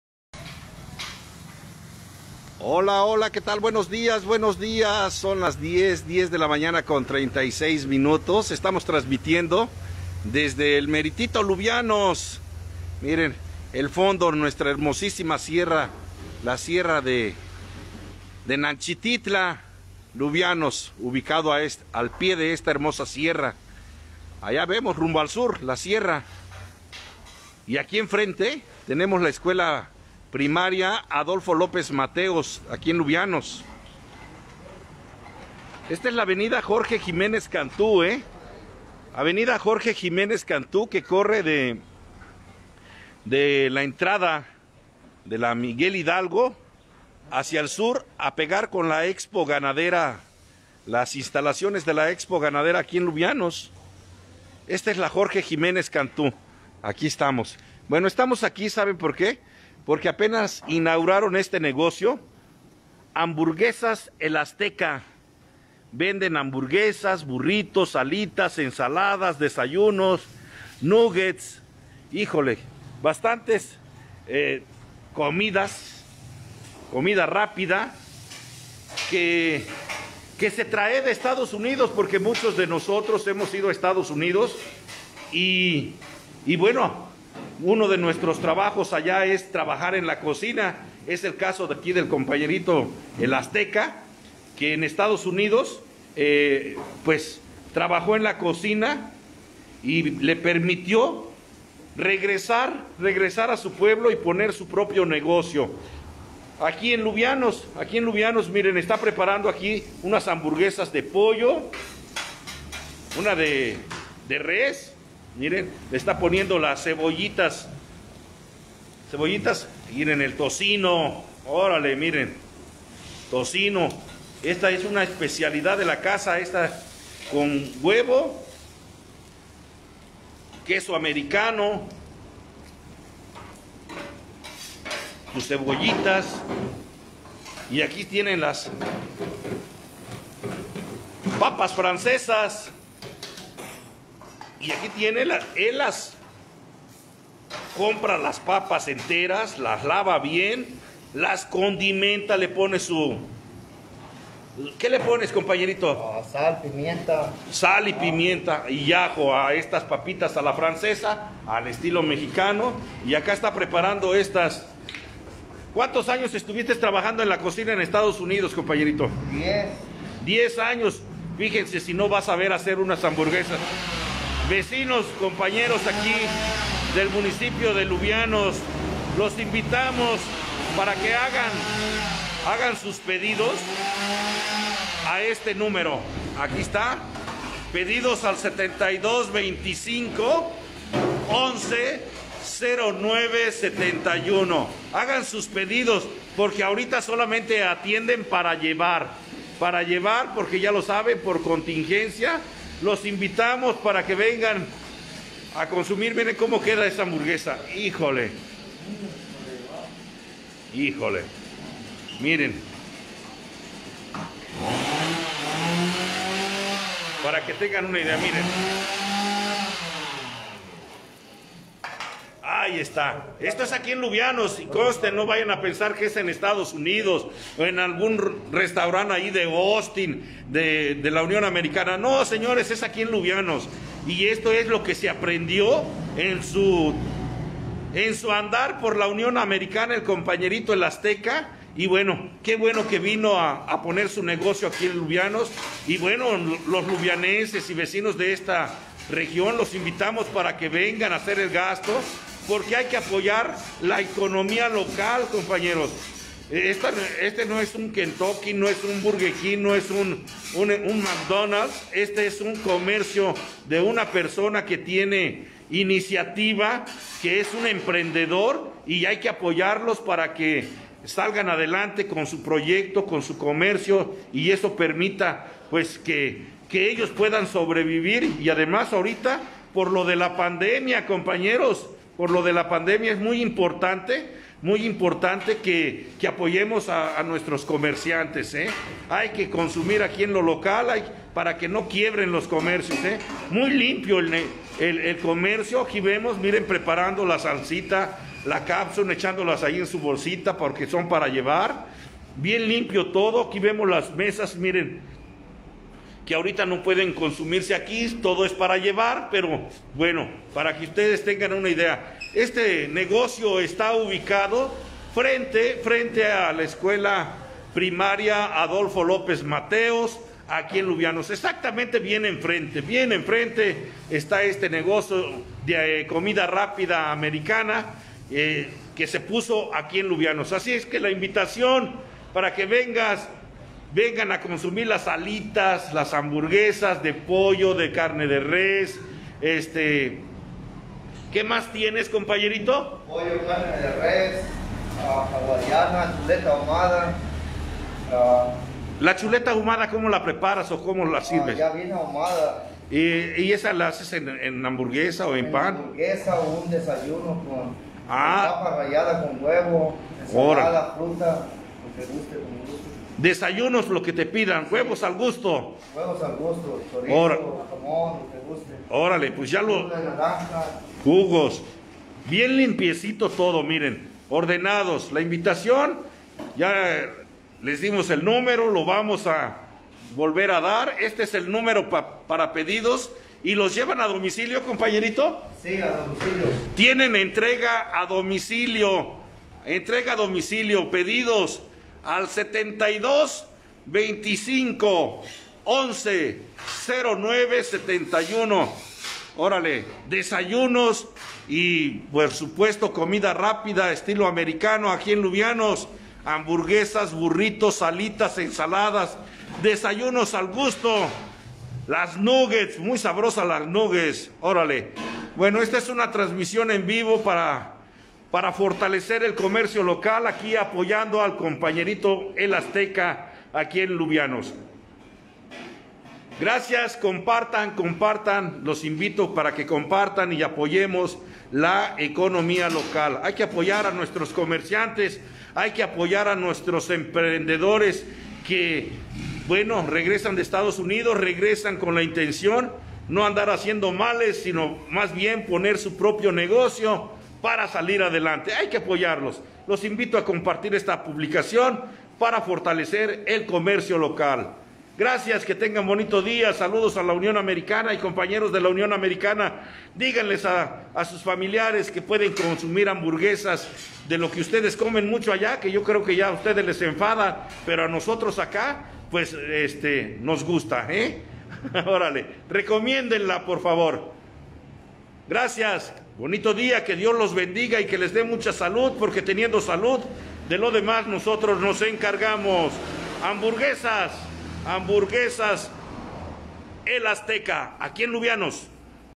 HAMBURGUESAS EL AZTECA, EN LUVIANOS.
HAMBURGUESAS EL AZTECA.
UBICADO SOBRE AV. JORGE JIMÉNEZ CANTU, FRENTE ALA ESCUELA PRIMARIA ADOLFO LÓPEZ MATEOS, LUVIANOS ESTADO DE MÉXICO.
ABIERTO LOS 7 DÍAS DE 8 A 6 DE LA TARDE.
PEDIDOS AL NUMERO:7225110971
CON SERVICIO SOLO PARA LLEVAR POR CONTINGENCIA.